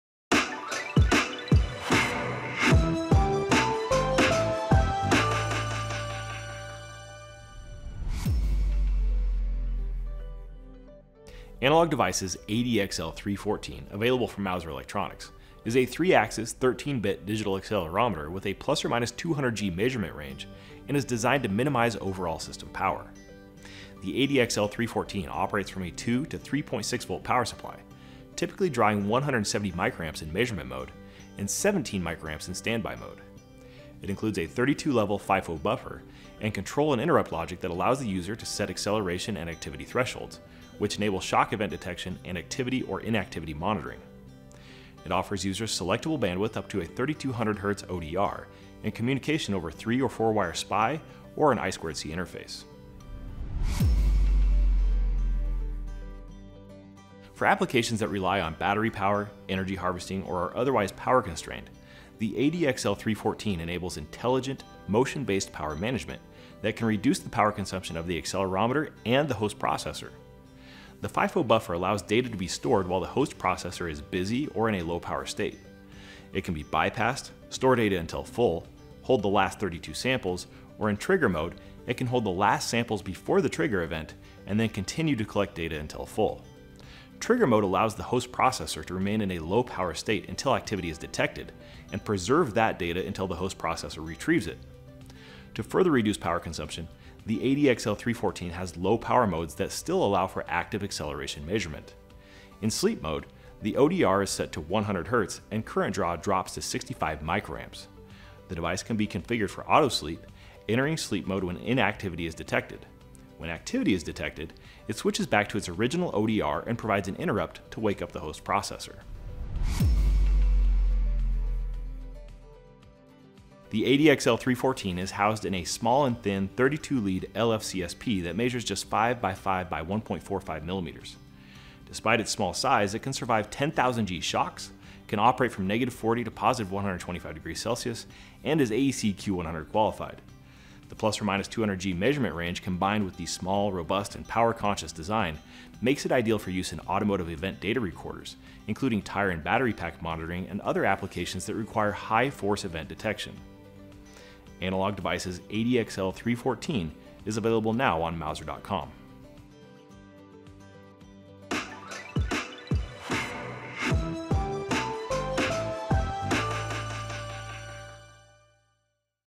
Analog Devices Inc. ADXL314 ±200g 3-Axis Digital Accelerometer - 3 for 3 | Mouser Electronics
Discover the exceptional capabilities of the Analog Devices Inc. ADXL314 ±200g 3-Axis Digital Accelerometer in our latest "3 for 3" video. This advanced accelerometer, available from Mouser Electronics, delivers accurate and reliable digital data output in a compact and power-efficient design.

The ADXL314 is a 3-axis digital accelerometer with a measurement range of ±200g and 13-bit resolution. It operates from a 2 to 3.6 volt power supply, consuming only 170 microamps in measurement mode and 17 microamps in standby mode. Equipped with a 32-level FIFO buffer and control and interrupt logic, the ADXL314 allows users to set acceleration and activity thresholds for shock event detection and activity monitoring. It offers selectable bandwidth options up to 3200 hertz ODR and supports communication via 3- or 4-wire SPI or I2C digital interfaces.

Designed for power-constrained applications, the ADXL314 enables intelligent motion-based power management, reducing power consumption for both the accelerometer and the host processor. The FIFO buffer efficiently stores data while the host processor is busy or in a low-power state, with options to bypass, hold the last samples, or operate in trigger mode. Trigger mode allows the host processor to remain in a low-power state until activity is detected, preserving important data until retrieval.

With its small and thin 32-lead LFCSP package measuring just 5x5x1.45 millimeters, the ADXL314 offers robustness and durability. It can withstand 10,000g shocks and operates reliably in a wide temperature range from -40 to +125 degrees Celsius. AEC-Q100 qualification ensures its suitability for automotive event data recorders, including tire and battery back monitoring, as well as other applications that require high-force event detection.

Upgrade your applications with the Analog Devices ADXL314 ±200g 3-Axis Digital Accelerometer, now available on Mouser.com. Experience precise measurement, power efficiency, and robust design in a compact package.

00:08 - What is the Analog Devices ADXL314?
00:53 - How is motion-based power management used?
01:59 - Where is the ADXL314 used?